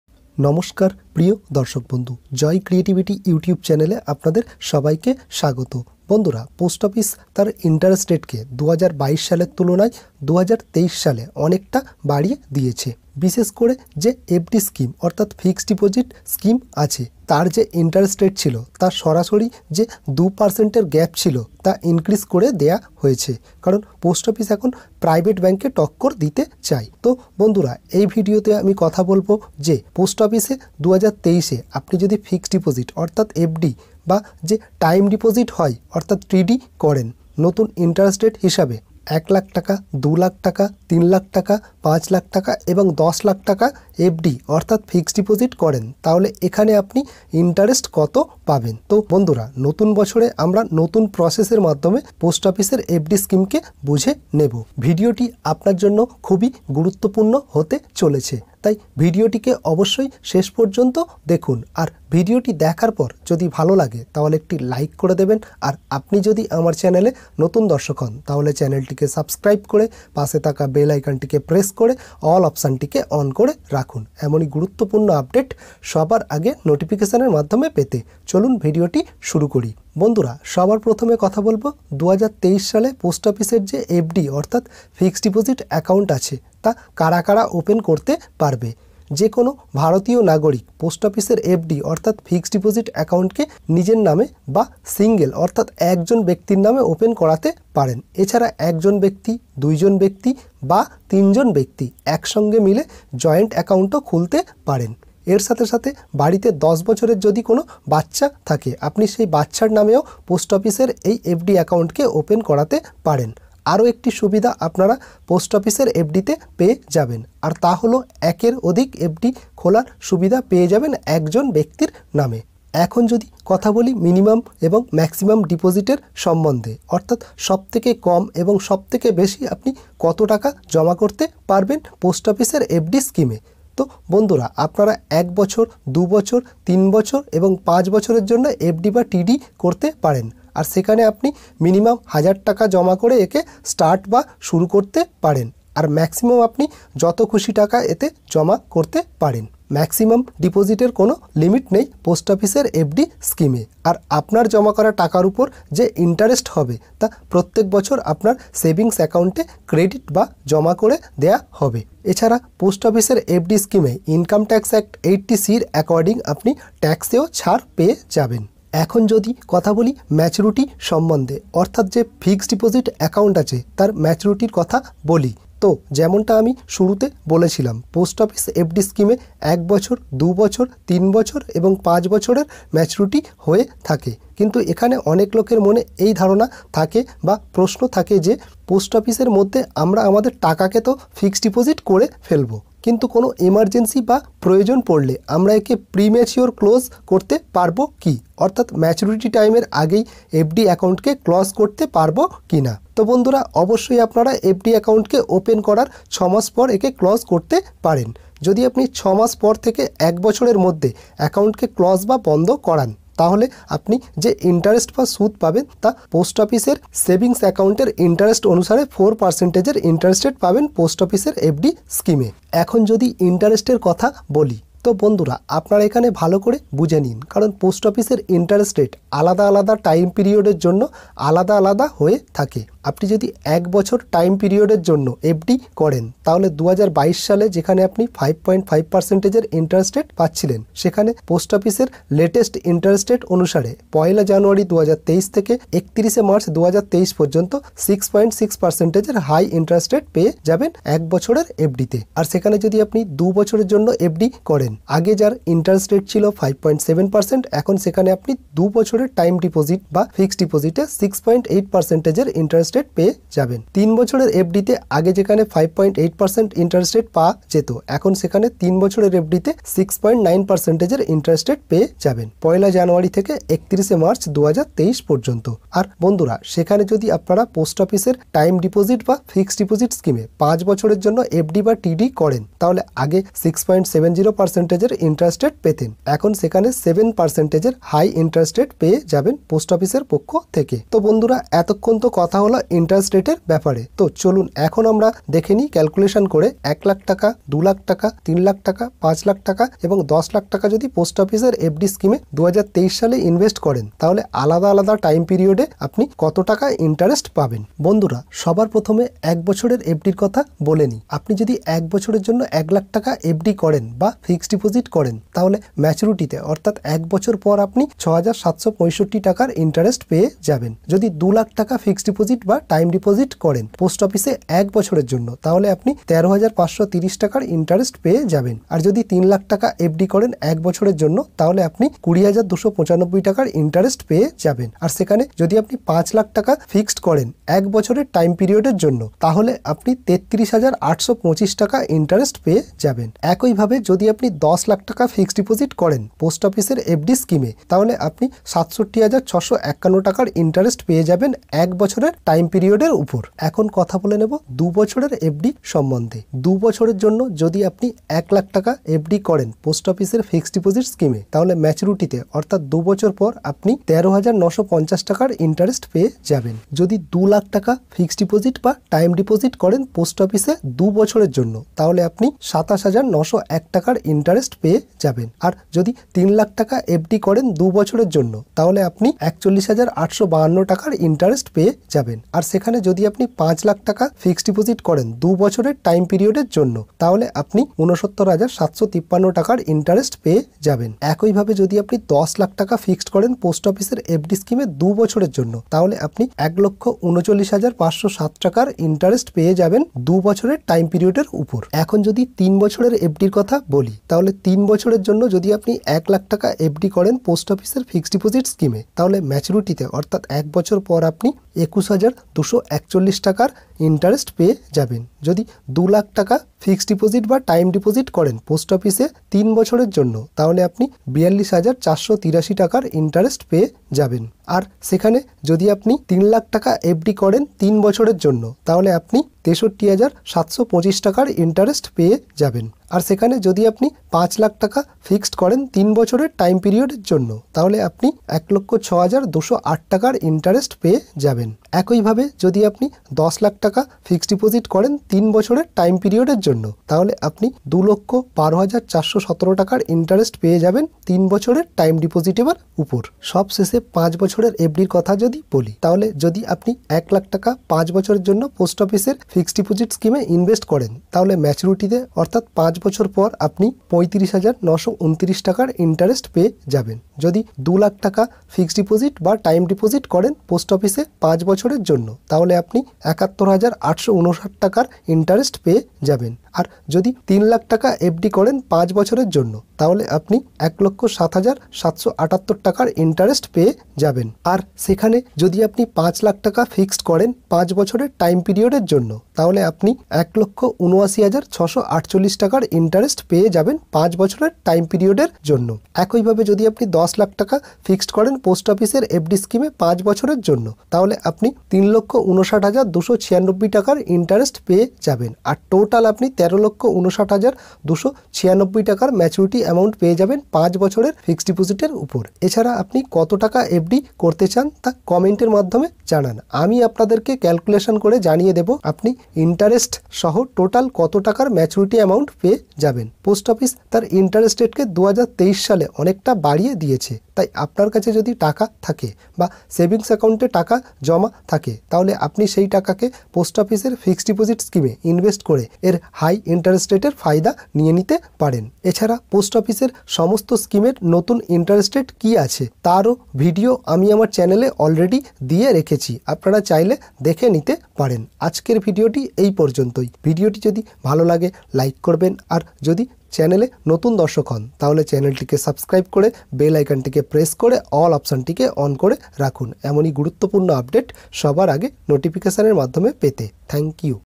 পোস্ট অফিস 2023 | FD-তে 7.25% সুদ | Post Office Fixed Deposit Scheme. Post Office Interest Rate 2023
Joy Creativity
Post office is now giving very high
interest rate on FD in 2023,
from which you can earn a
lot of interest money.

Category - Education